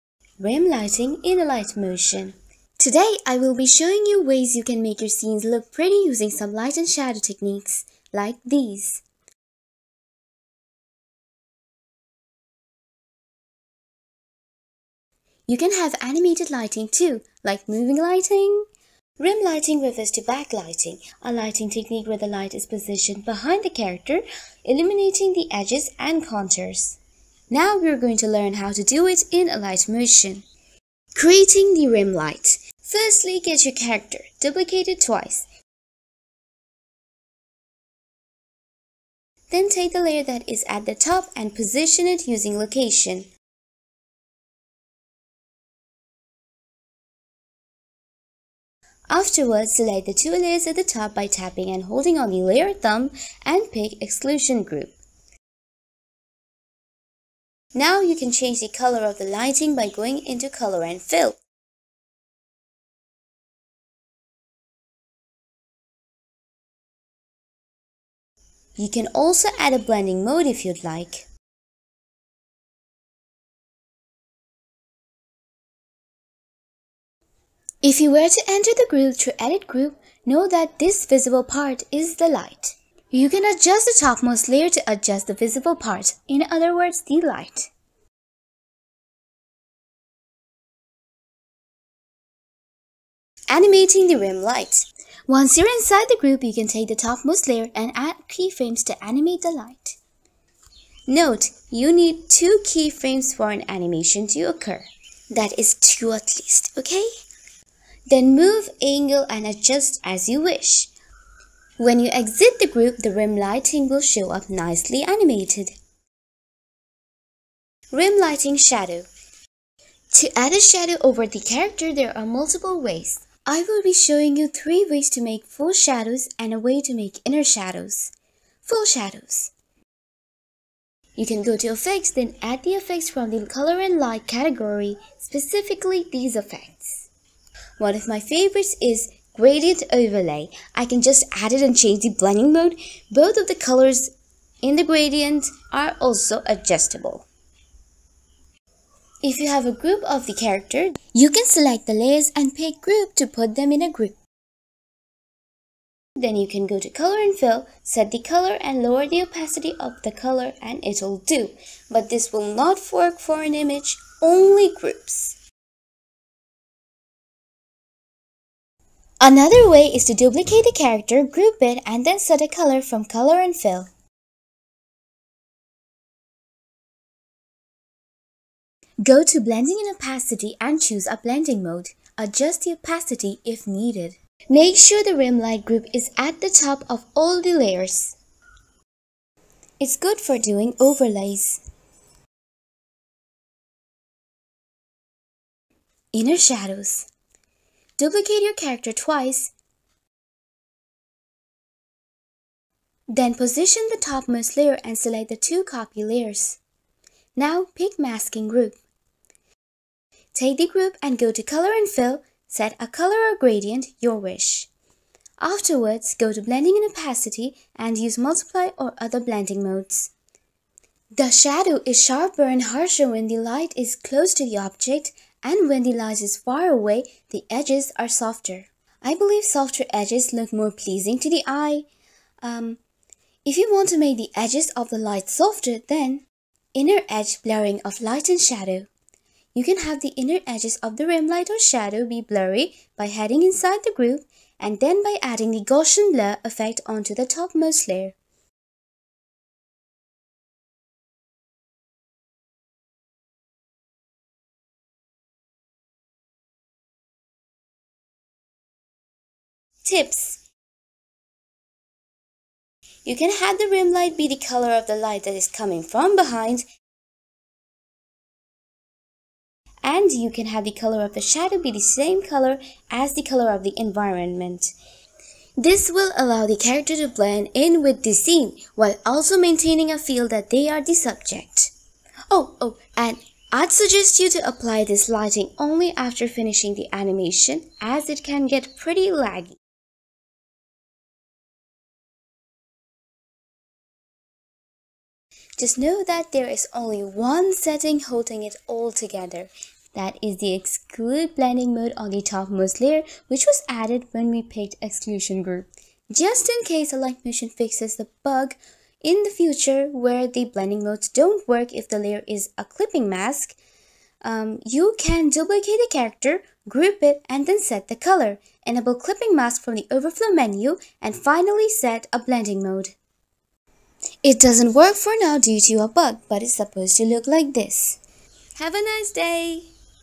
Rim Lighting Tutorial in Alight Motion [FREE]
These lighting take only ONE minute to implement once you learn it!

✦ Chapters:
00:00 Intro + Result
00:31 Creating the Rim Light
01:33 Animating the Rim Light
01:57 Rim Light Shadows
02:08 Full Shadow 1
02:29 Full Shadow 2
02:48 Full Shadow 3
03:15 Inner Shadows (POPULAR)
03:56 Inner Blurring of Light & Shadows (Cool Effect)
04:19 Tips
04:54 The Secret
05:06 Clipping Mask Shadow (BUG)

✦ Alternative Titles:
Rim Lighting Tutorial
Lighting Tutorial Easy
Animated Lighting
Beautiful Lighting
Cinematic Lighting
Alight Motion